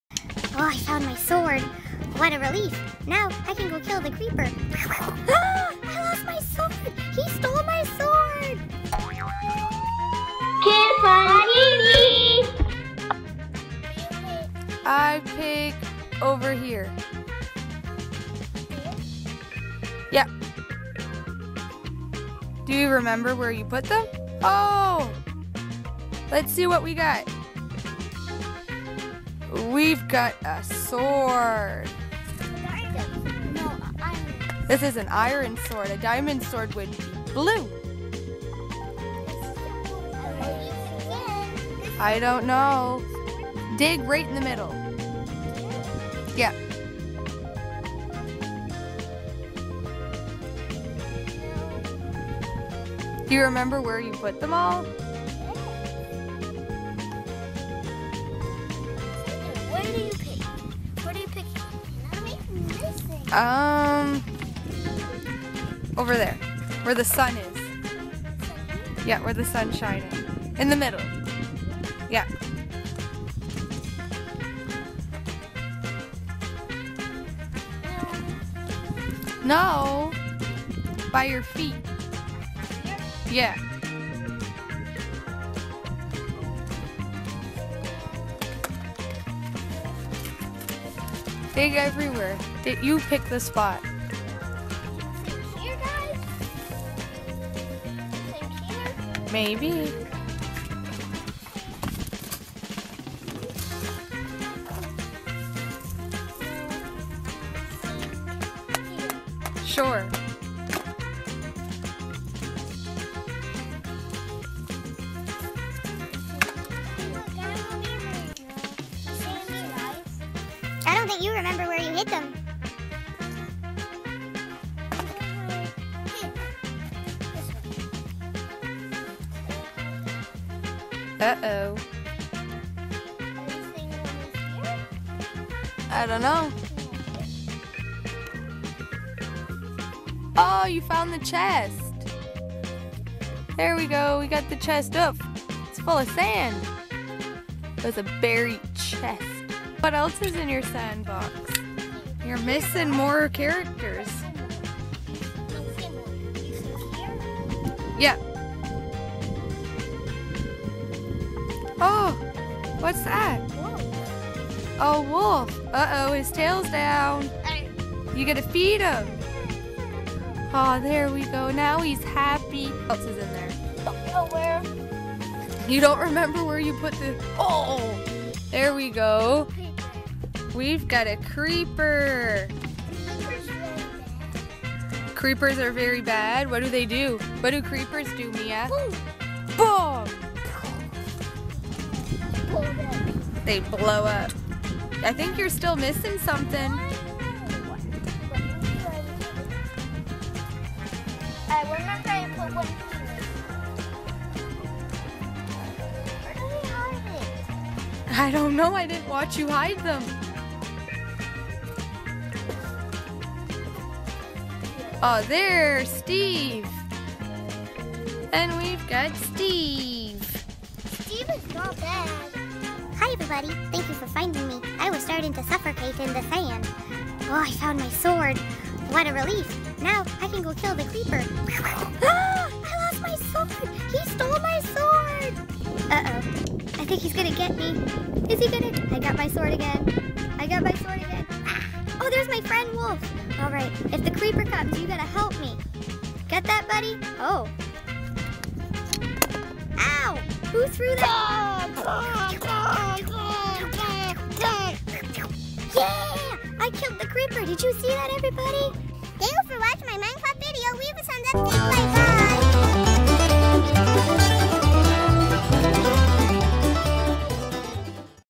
Minecraft surprise Hidden in the Sand / Steve VS Creeper / Surprise video / KidsFunTV
Hey everyone ,
This entire video was made up on an idea my son had. He came up with the entire video.